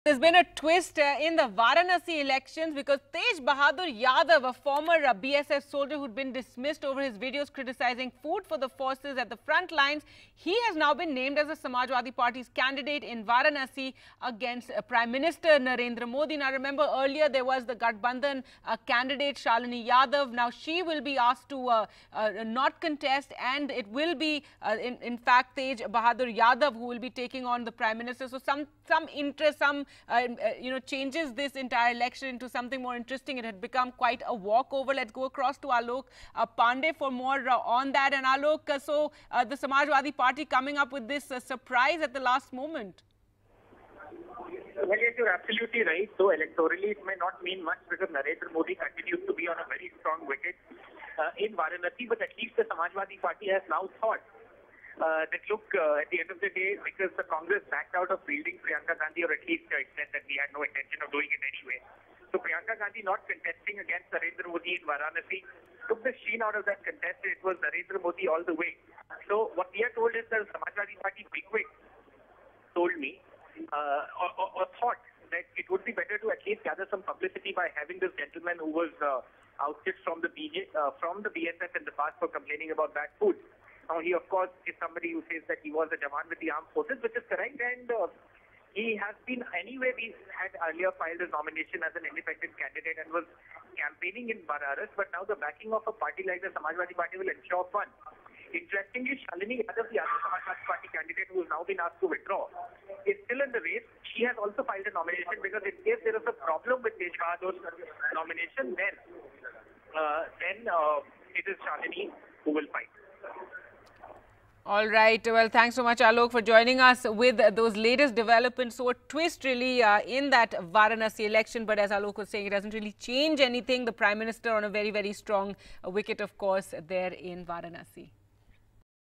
Sacked Soldier Tej Bahadur Yadav Named Samajwadi's Varanasi Candidate
Tej Bahadur Yadav, the former Border Security Force (BSF) soldier dismissed over his videos criticizing food for the forces at the frontlines, has been named the Samajwadi Party's candidate in Varanasi against Prime Minister Narendra Modi.